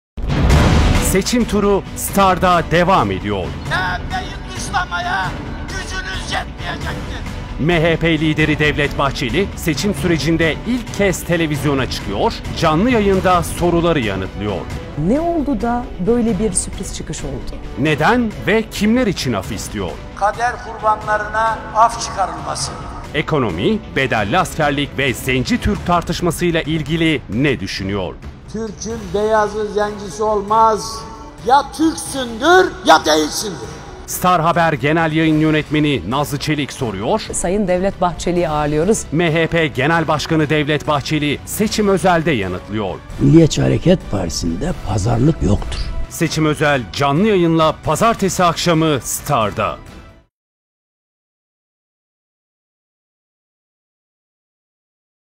TÜRKİYE EKRAN BAŞINA
Genel Başkanımız Devlet BAHÇELİ Bugün, StarTV - NTV ortak yayınıyla SeçimÖzel Programında Nazlı Çelik'in Konuğu olacaktır.